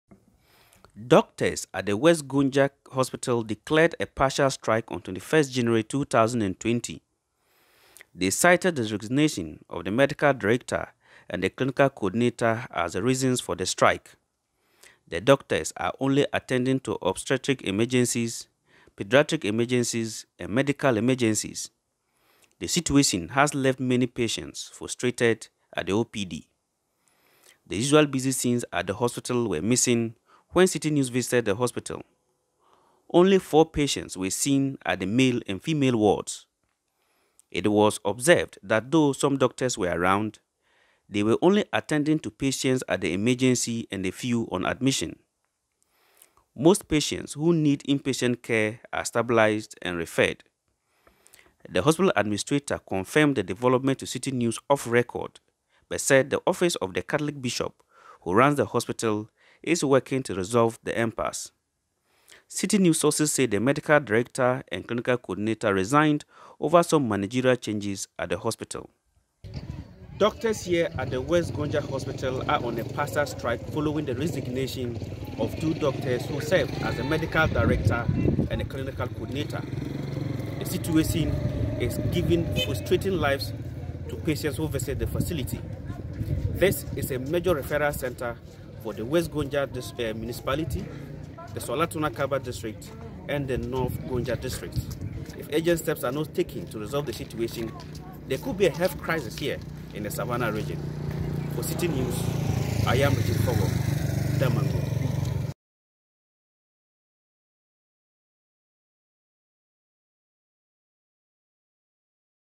Strike by medical doctors at West Gonja Hospital intensifies | Citi Newsroom
Wards at the West Gonja Hospital in Damongo are almost empty as a partial strike by doctors intensifies.

Doctors at the hospital began a partial strike on 21st January 2022, following the resignation of the Medical Director and Clinical Coordinator.